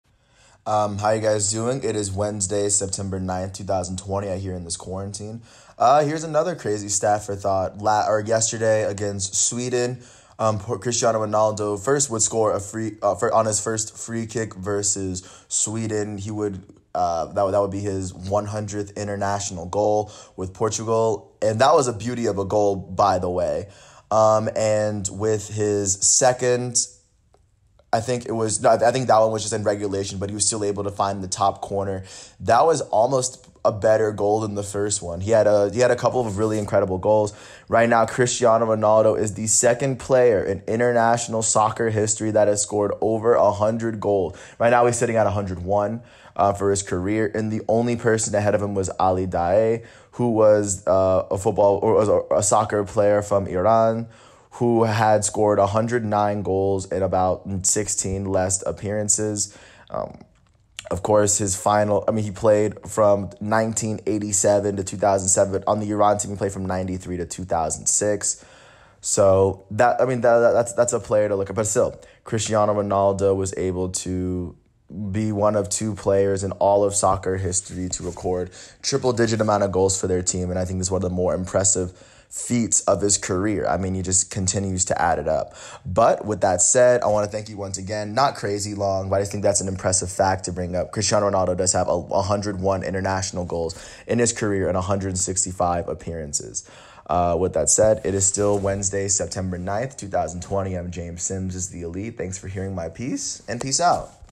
"Elite" Fact: Cristiano Ronaldo Scores #101
Recorded September 9, 2020. The picture was taken September 9, 2020, so the sports stories and topics are consistent with this date. This is my 4th "Elite" Fact that I made where I drop a fact that blew my mind and would probably blow the minds of most sports fans. My intent with these facts is to give people a sense of how incredible it is to watch a player who has scored over 100 international goals for Portugal for the last 17 years. I recorded this episode and took this picture with my iPhone. Thanks for being a part of the journey.